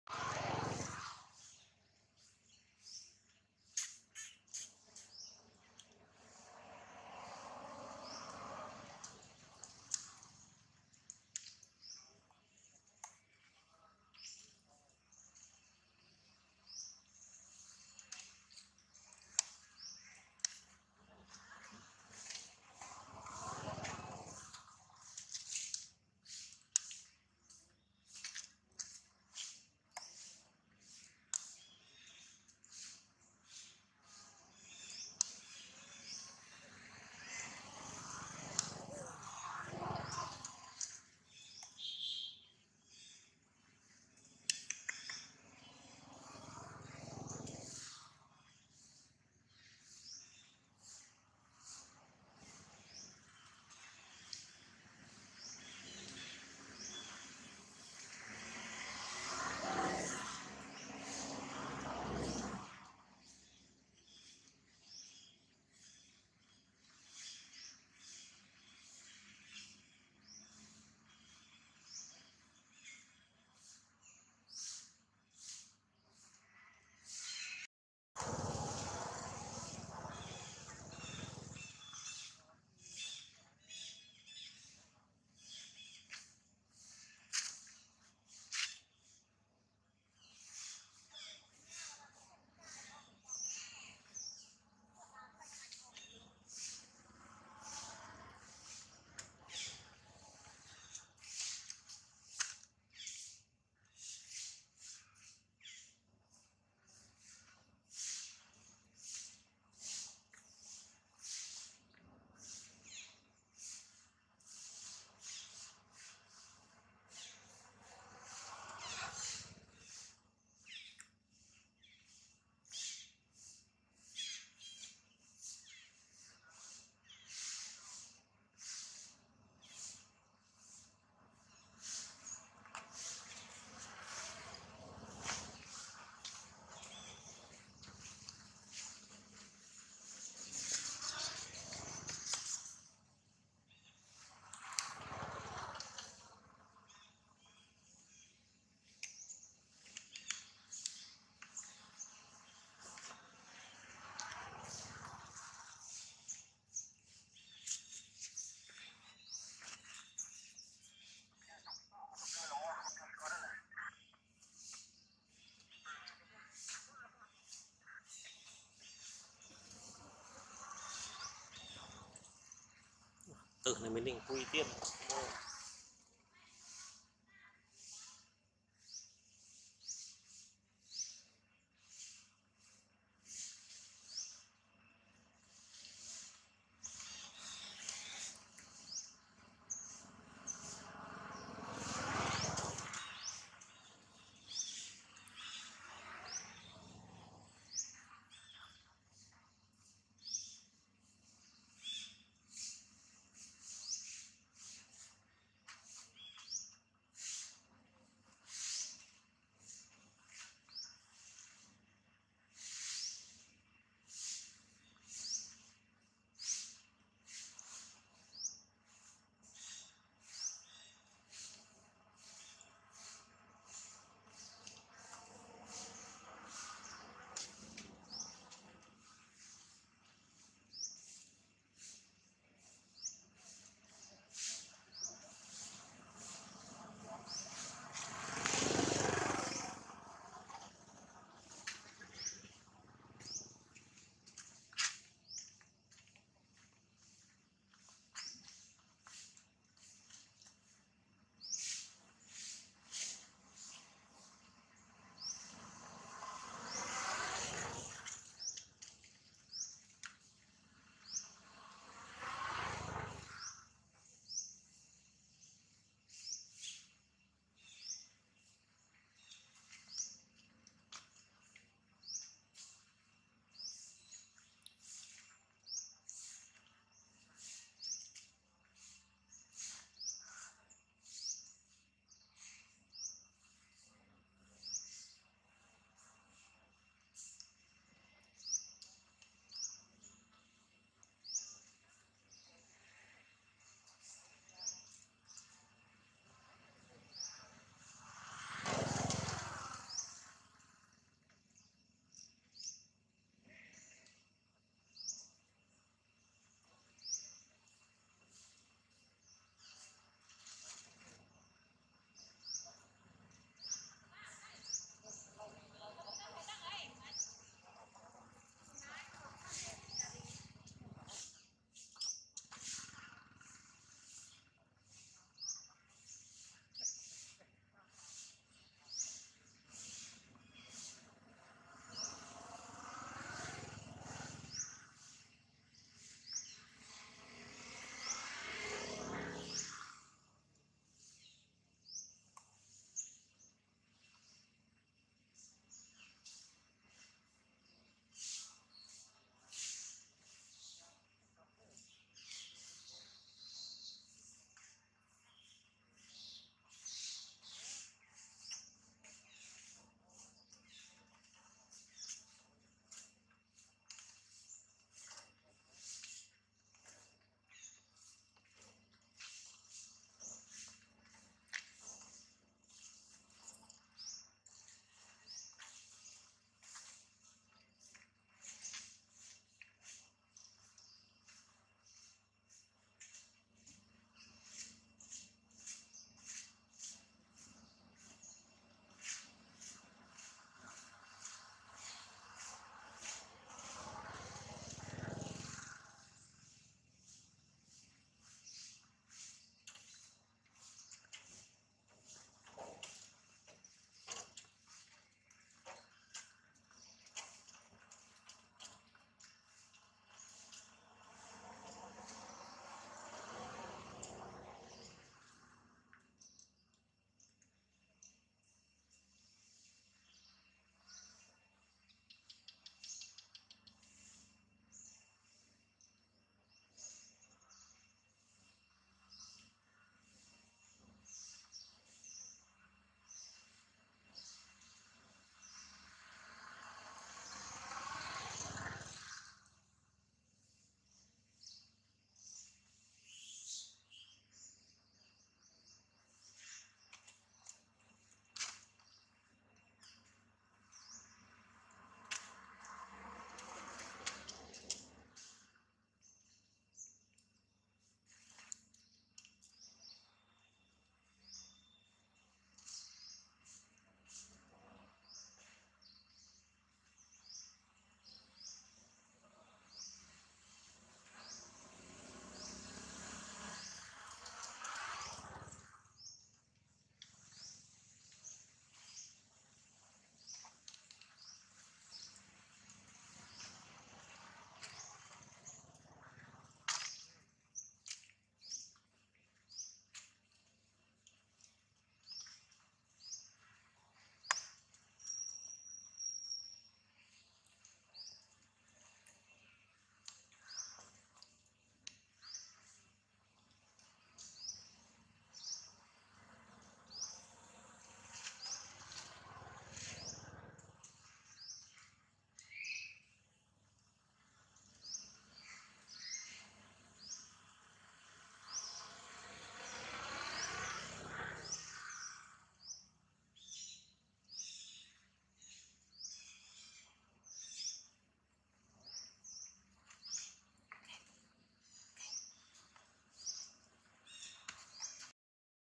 HURT FULL ! STRONG PUSH ! Pregnant mom Jane look like born her new baby soon,jane try to push strong
Everyday Monkey